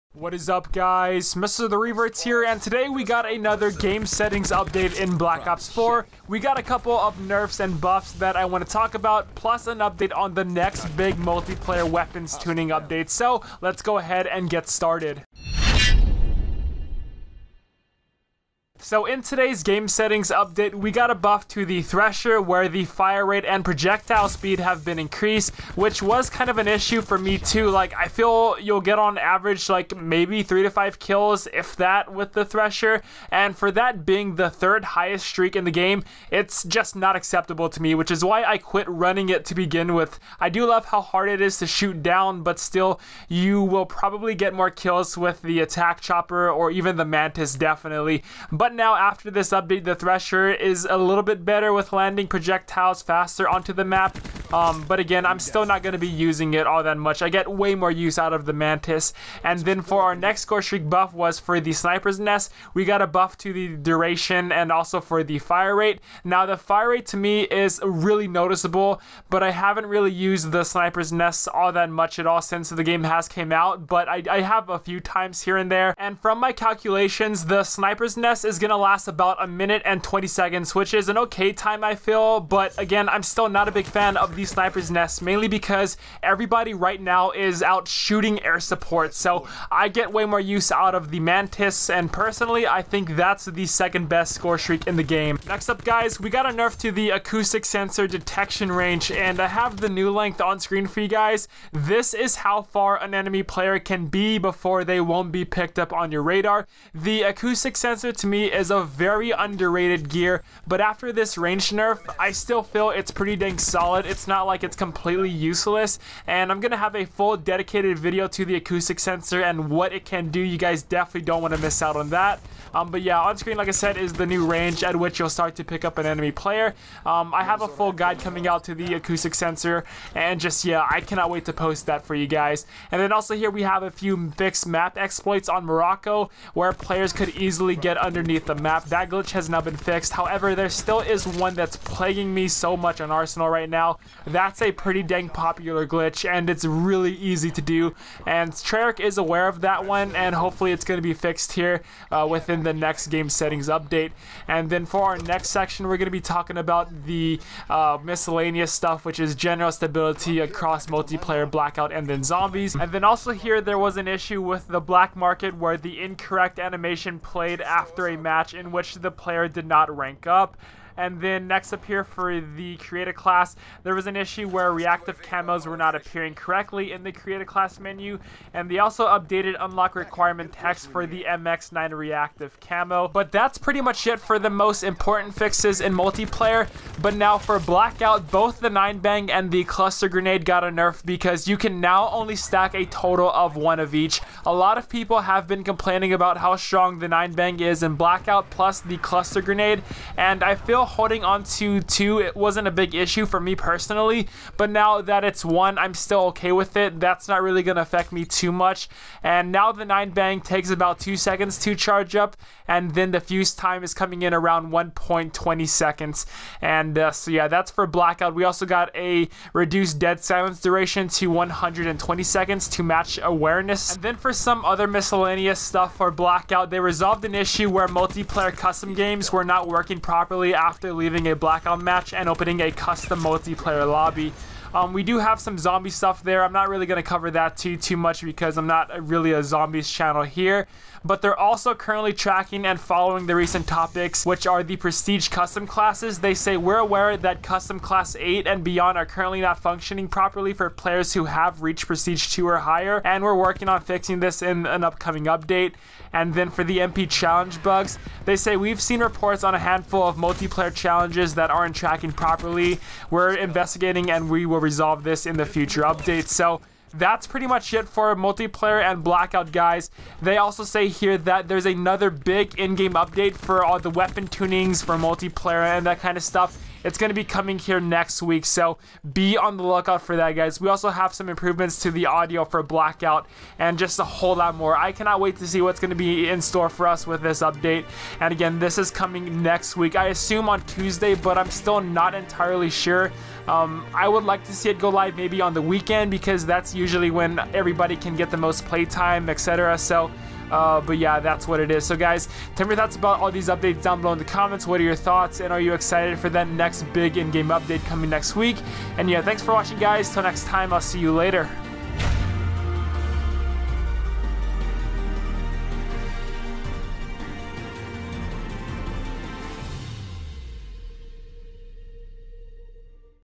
Black Ops 4: MAJOR Weapons Update Coming - 9-Bang Nerf In Blackout & More!
Black Ops 4: MAJOR Weapons Update Coming - 9-Bang Nerf In Blackout & More! Yet another game settings update came out today for black ops 4 w/ some fairly decent nerfs and buffs to scorestreaks, the 9bang, and more. Plus we have another massive weapon tuning update coming next week with some more surprises in store.

Oct. 24 Update: Scorestreak and Blackout Tuning, Stability Improvements